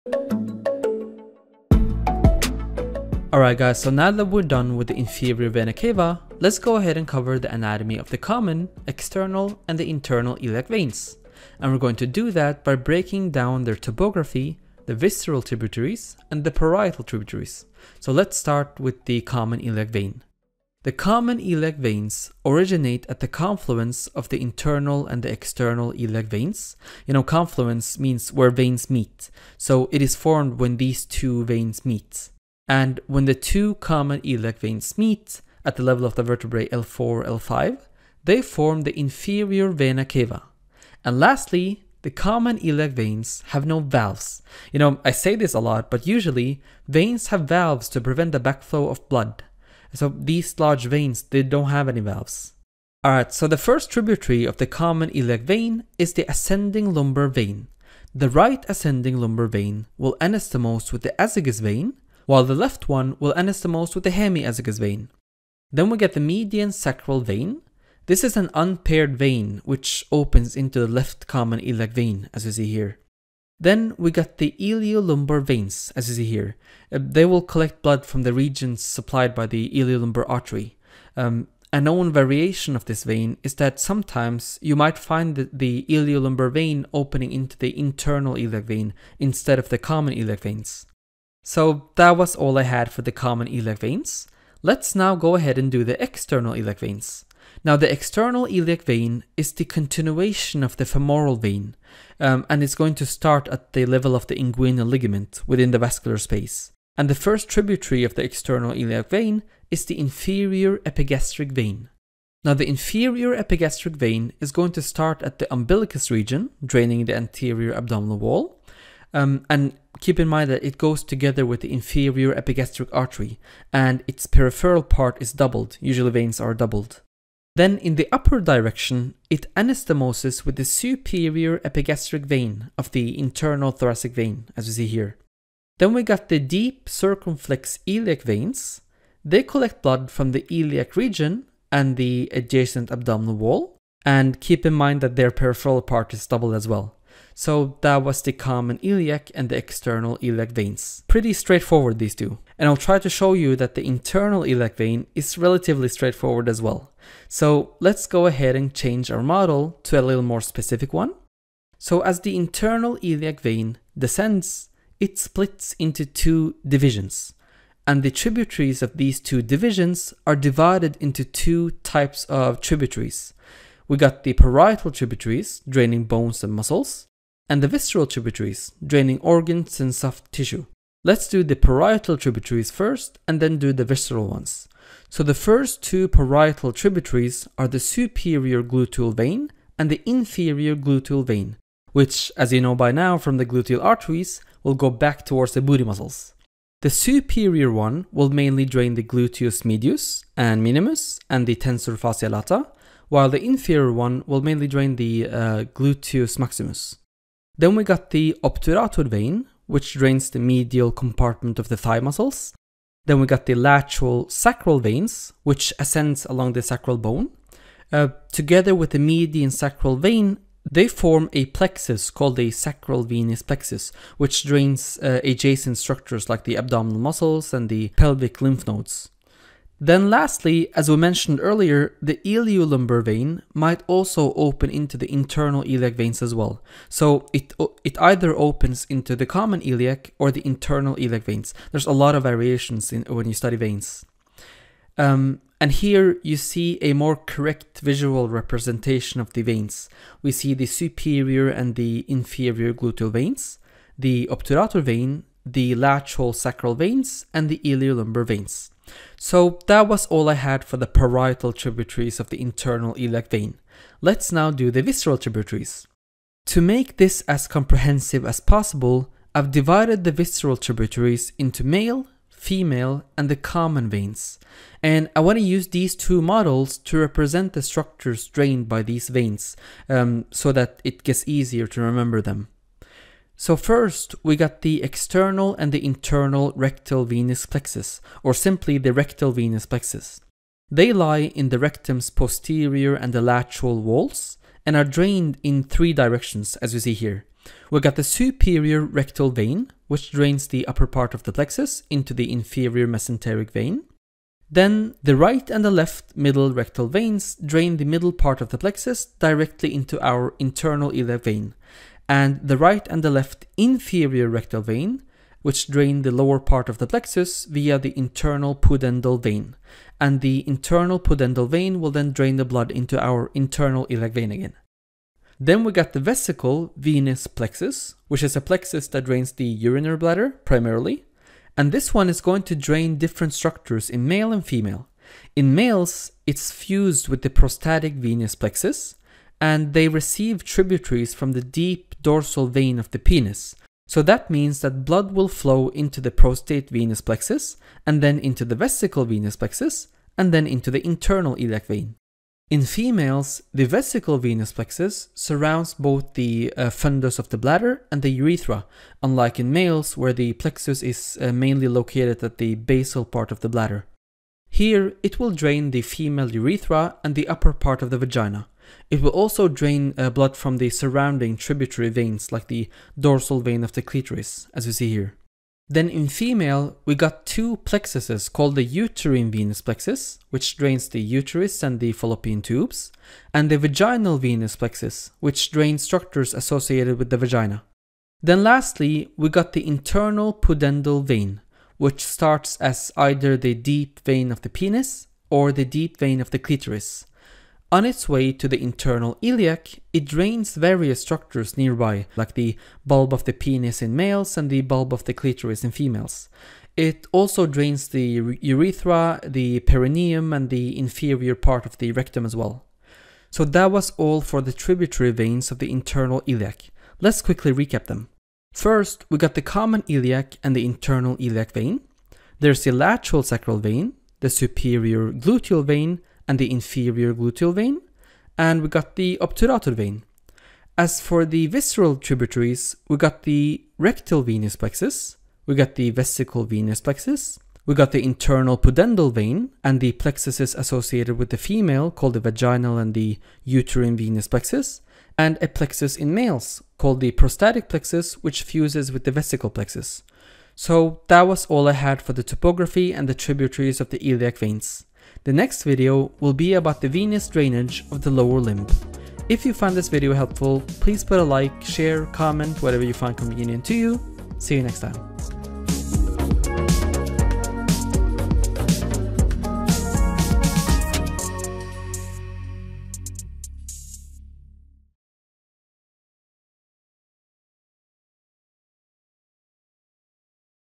Iliac Veins Scheme (Visceral + Parietal Tributaries) | Anatomy
Description
0:00 Introduction
0:10 Content
0:19 Common Iliac Vein
1:34 External Iliac Vein
2:33 Internal Iliac Vein
3:00 Parietal Tributaries of Internal Iliac Vein
4:33 Visceral Tributaries of Internal Iliac Vein
7:30 Ending

Overview of the anatomy of the common, external, and internal iliac veins, their tributaries, and their roles in the vascular system.

Common Iliac Vein:
Formed by the confluence of the internal and external iliac veins at the level of L4-L5 vertebrae.
No valves present in the common iliac veins.
Tributaries:
- Ascending lumbar vein (anastomoses with azygos and hemiazygos veins).
- Median sacral vein (opens into the left common iliac vein).
- Iliolumbar veins (could open into the internal iliac vein).

External Iliac Vein:
Continuation of the common femoral vein starting at the inguinal ligament.
Tributaries:
- Inferior epigastric vein (drains the anterior abdominal wall, anastomoses with superior epigastric vein).
- Deep circumflex iliac veins (collects blood from the iliac region).

Internal Iliac Vein:
Divided into two divisions with parietal and visceral tributaries.
Parietal Tributaries:
- Superior and inferior gluteal veins (drain gluteal muscles).
- Obturator vein (drains medial thigh muscles).
- Lateral sacral veins (form sacral venous plexus).
Visceral Tributaries:
Categorized into male, female, and others.
- Rectal Venous Plexus: Drained by superior, middle, and inferior rectal veins.
- Vesical Venous Plexus: Drains urinary bladder.
  Male: Fuses with prostatic venous plexus.
  Female: Surrounds bladder and drains urethra.
- Uterine and Vaginal Venous Plexuses (female).
- Internal Pudendal Vein: Drains various structures near the pelvis.

Veins included in this video:
Common Iliac Vein
External Iliac Vein
Internal Iliac Vein
Inferior Vena Cava
Ascending Lumbar Vein
Azygos Vein
Hemiazygos Vein
Median Sacral Vein
Iliolumbar Veins
Inguinal Ligament
Inferior Epigastric Vein
Superior Epigastric Vein
Deep Circumflex Iliac Vein
Superior Gluteal Vein
Inferior Gluteal Vein
Obturator Vein
Lateral Sacral Vein
Sacral Venous Plexus
Rectal Venous Plexus
Superior Rectal Vein
Middle Rectal Vein
Inferior Rectal Vein
Internal Pudendal Vein
Vesical Venous Plexus
Prostatic Venous Plexus
Deep Dorsal Vein of the Penis
Dorsal Vein of the Clitoris
Uterine Venous Plexus
Vaginal Venous Plexus